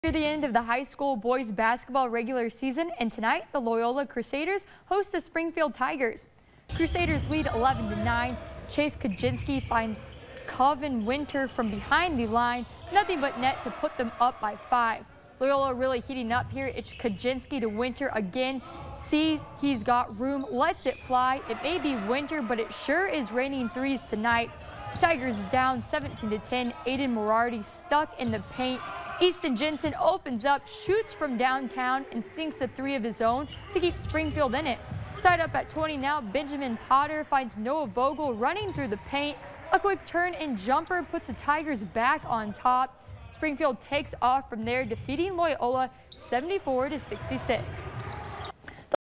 Springfield takes down Loyola boys basketball in regular season finale
Springfield will host Madelia and Loyola will host GHEC/T/ML in the Section 2A tournament March 5.